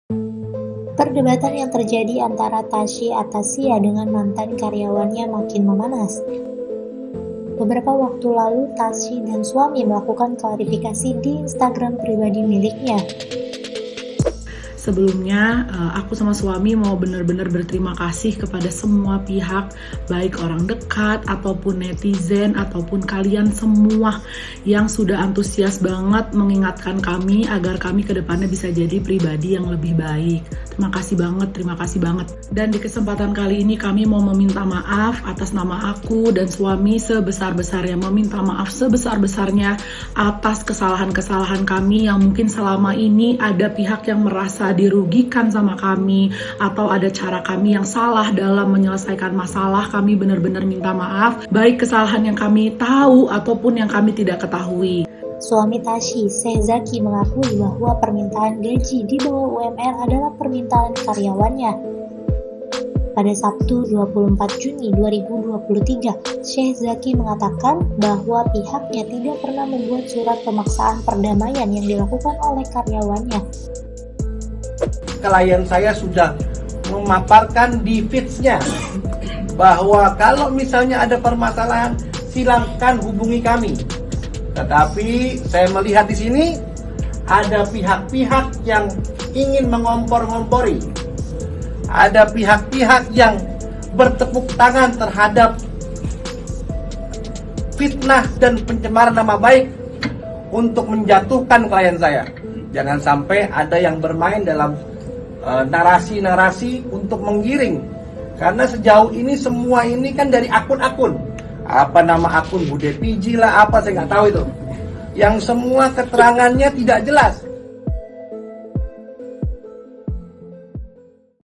Suami Tasyi Athasyia Buka Suara
Perdebatan yang terjadi antara Tasyi Athasyia dengan mantan karyawannya makin memanas.

beberapa waktu lalu, Tasyi dan suami melakukan klarifikasi di instagram pribadi miliknya.

Suami Tasyi, Syech Zaki mengakui bahwa permintaan gaji dibawah UMR adalah permintaan karyawannya.

Pada Sabtu, 24 Juni 2023 Syech Zaki mengatakan bahwa pihaknya tidak pernah membuat surat pemaksaan perdamaian yang dilakukan oleh karyawannya.

Akun official  Info indonesia

iNOVASI untuk NEGERi